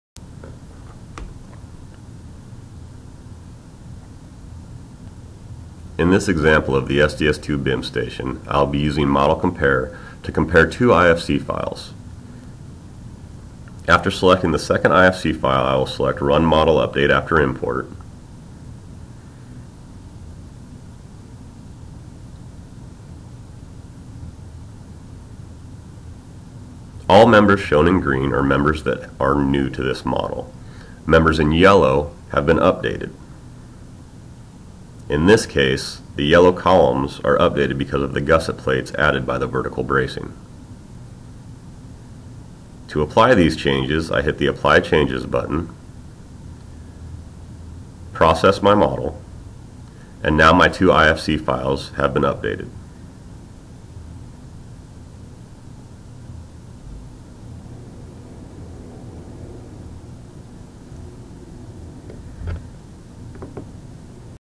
Using Model Compare in SDS/2 BIM
Model Compare allows users to compare IFC files for the same model to easily locate changes between models and easily updated by allowing the user to accept or reject the changes.